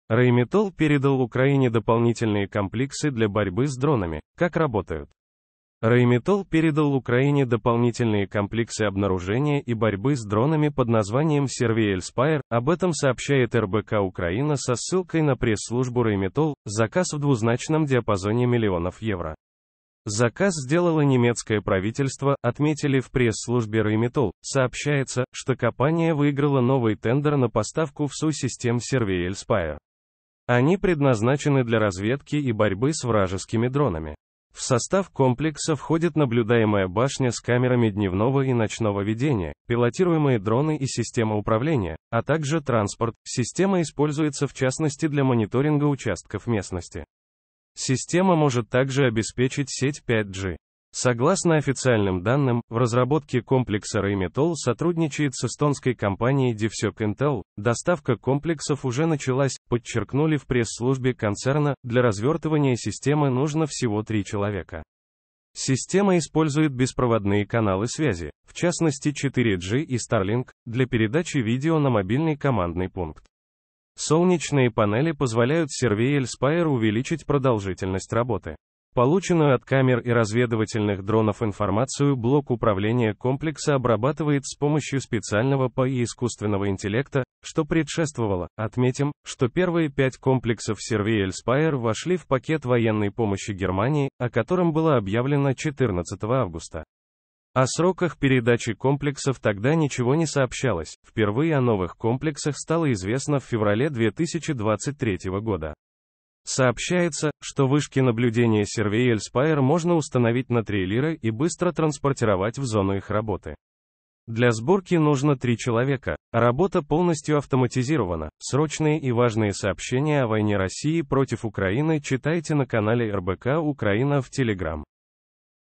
Rheinmetall передал Украине дополнительные комплексы для борьбы с дронами: как работают.
дрони Военнаяпомощь Rheinmetall передал Украине дополнительные комплексы обнаружения и борьбы с дронами под названием SurveilSpire. Об этом сообщает РБК-Украина со ссылкой на пресс-службу Rheinmetall. "Заказ в двузначном диапазоне миллионов евро. Заказ сделало немецкое правительство", – отметили в пресс-службе Rheinmetall. Сообщается, что копания выиграла новый тендер на поставку ВСУ систем SurveilSpire. Они предназначены для разведки и борьбы с вражескими дронами. В состав комплекса входит наблюдаемая башня с камерам